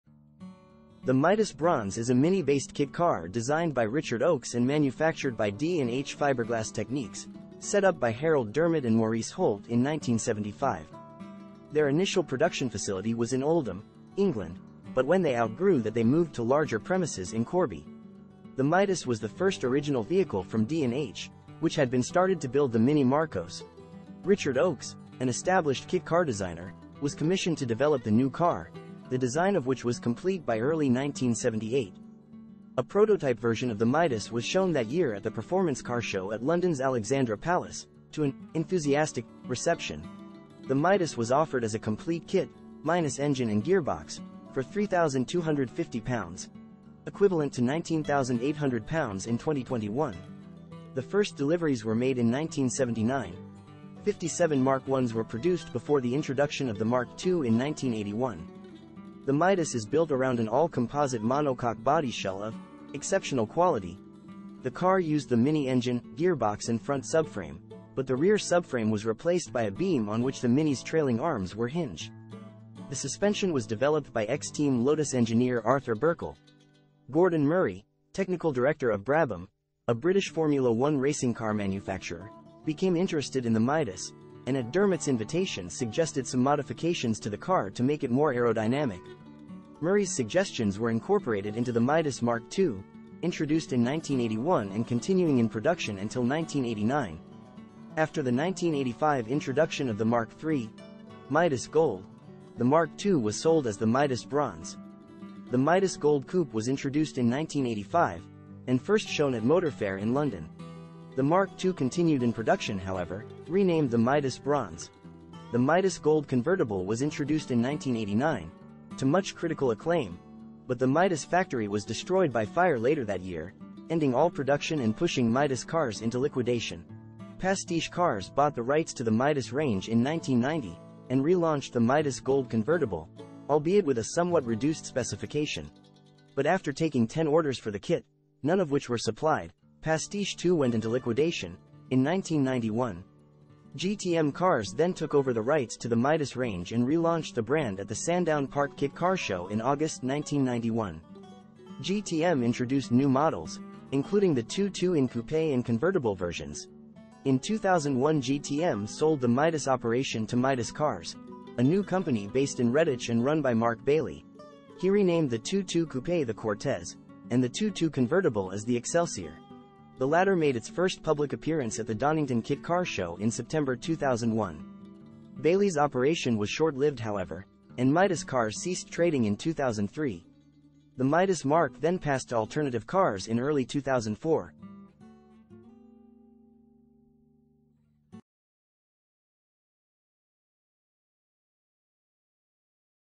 Midas Bronze
The Midas Bronze is a Mini-based kit car designed by Richard Oakes and manufactured by D&H Fibreglass Techniques, set up by Harold Dermott and Maurice Holt in 1975.